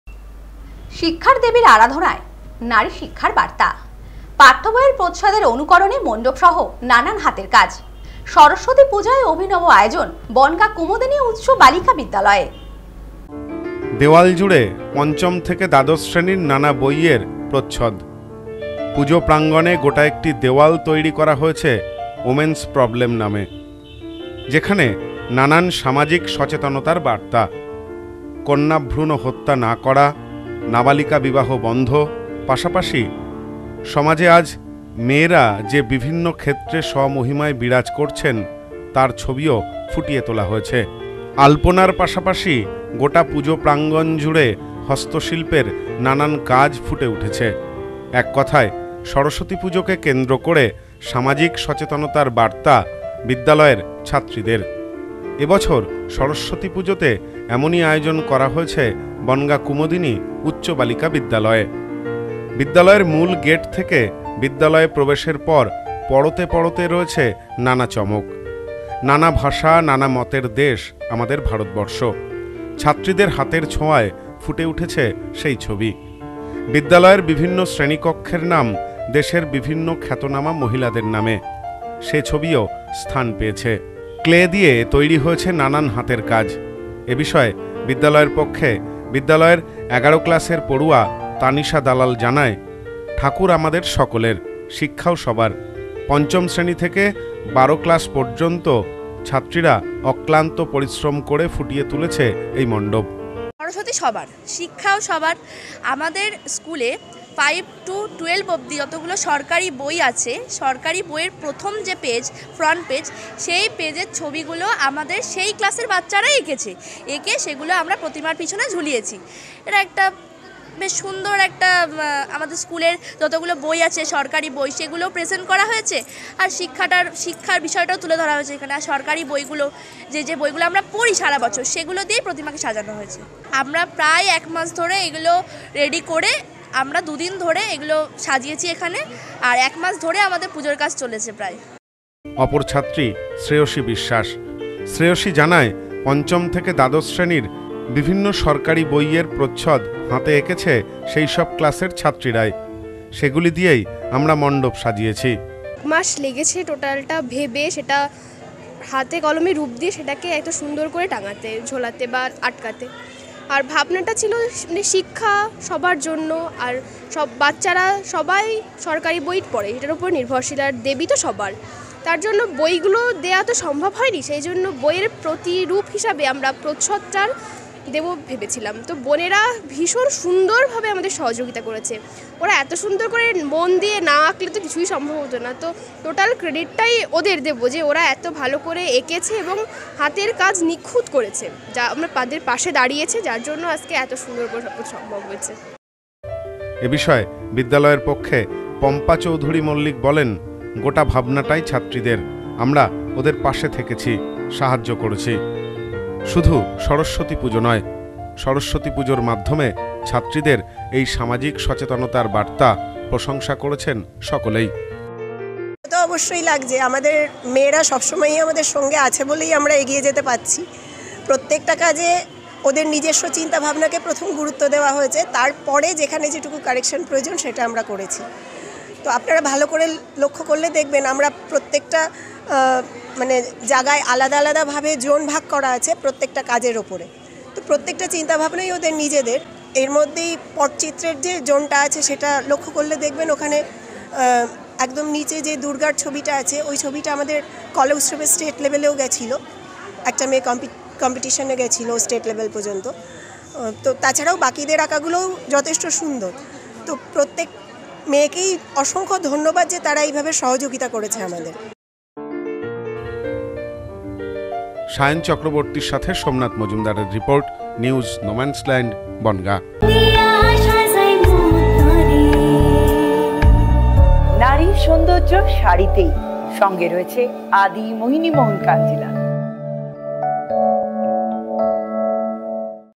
শিক্ষার দেবীর আরাধনায় নারী শিক্ষার বার্তা।পাঠ্য বইয়ের প্রচ্ছেদের অনুকরণে মন্ডপ সহ নানান হাতের কাজ।
শিক্ষার দেবীর আরাধনায় নারী শিক্ষার বার্তা।পাঠ্য বইয়ের প্রচ্ছেদের অনুকরণে মন্ডপ সহ নানান হাতের কাজ। সরস্বতী পুজোয় অভিনব আয়োজন বনগাঁ কুমুদিনী উচ্চ বালিকা বিদ্যালয়ে।